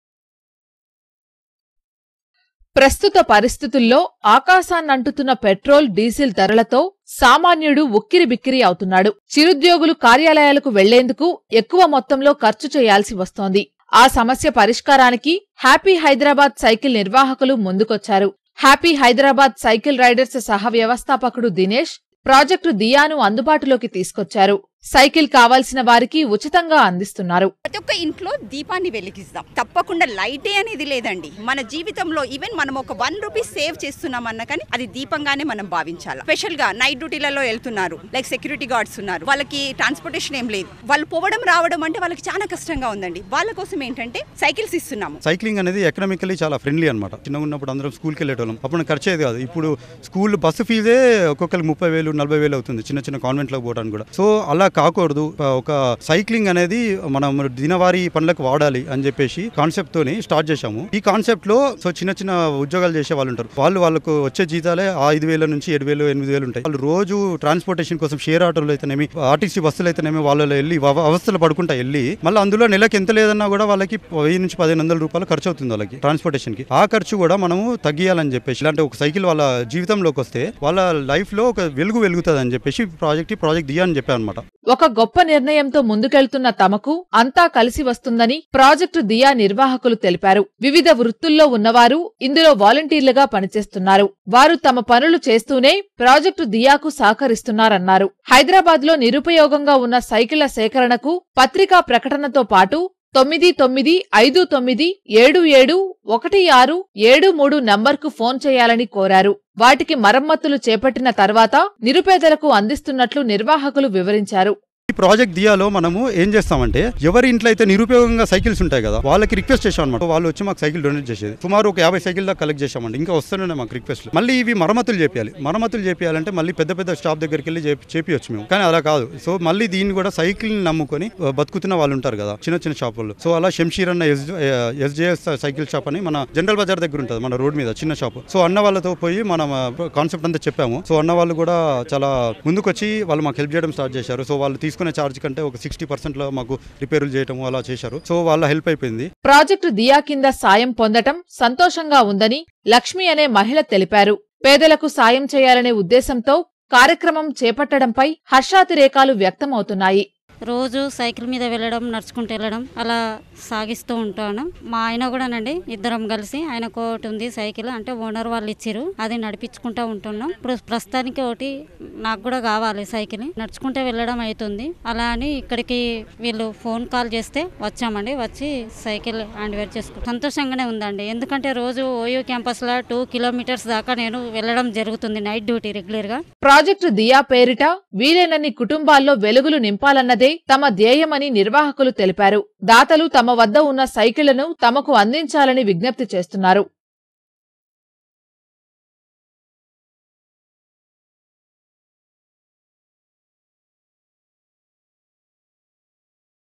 పేదలకు ఉచితంగా సైకిళ్లు | Project Diya : Happy Hyderabad Distributed Free Bicycles To Poor People
ప్రాజెక్టు దియా పేరుతో సైకిల్ కొనలేని వారికి ఉచితంగా అందజేస్తూ... వాళ్ల జీవితాల్లో హ్యాపీ హైదరాబాద్‌ సైకిల్‌ రైడర్‌ నిర్వాహకులు వెలుగులు నింపుతున్నారు. సైకిల్ అవసరం ఉన్న వారెవరైనాసరే ఒక్క ఫోన్ కాల్ చేస్తే సైకిల్ సమకూర్చుతున్నారు. నిరుపేదలు, చిరుద్యోగులే లక్ష్యంగా తమ సేవలను అందిస్తున్నారు.